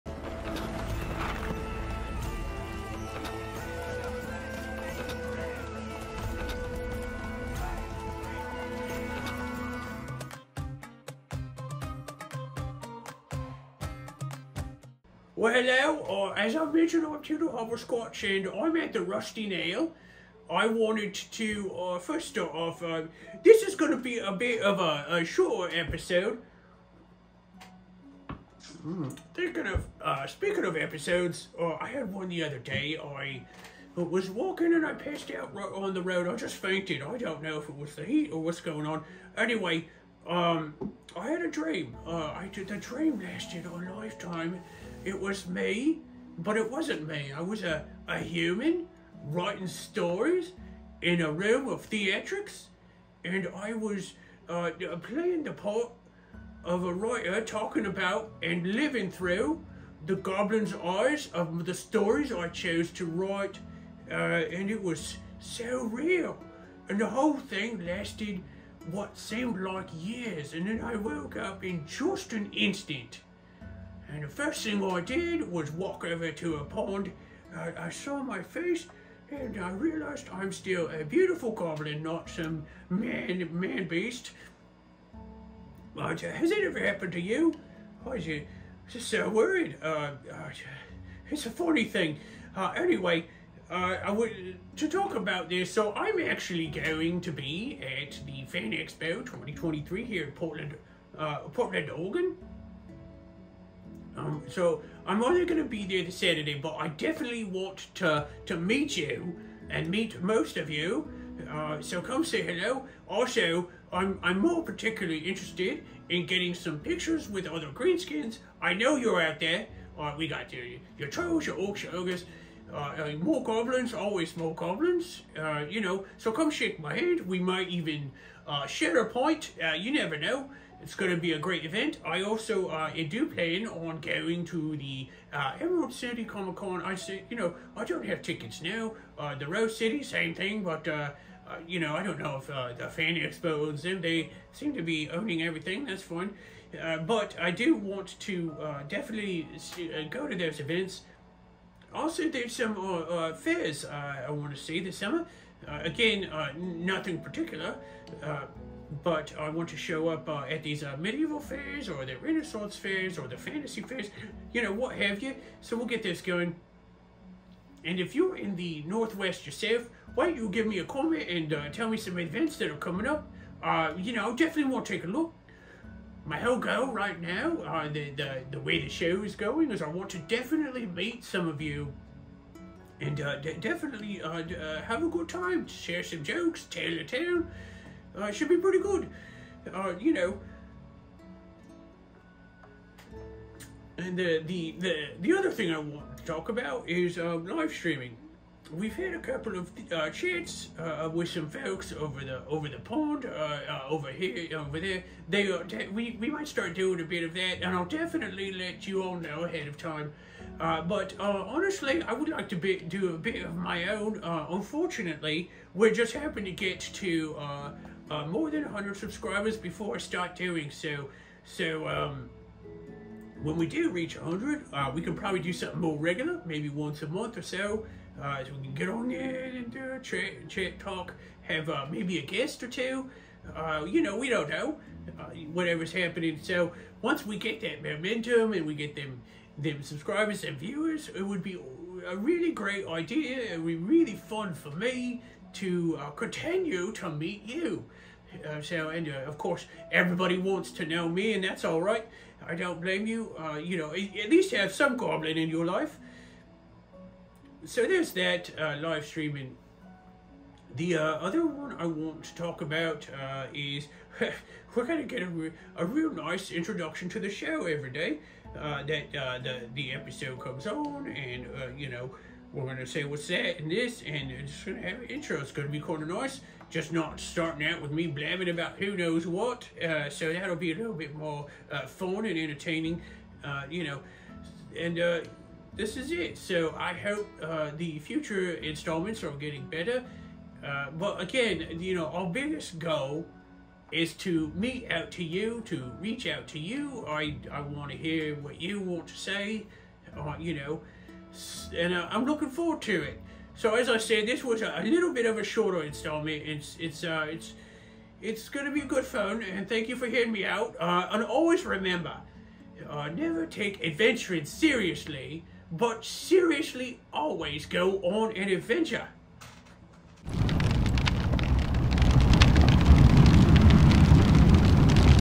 Upcoming Events 2023
Tildar Discusses Fan Expo, Renaissance Faires, And Much More In The Upcoming Lineup For The Life Of Hobblesquatch.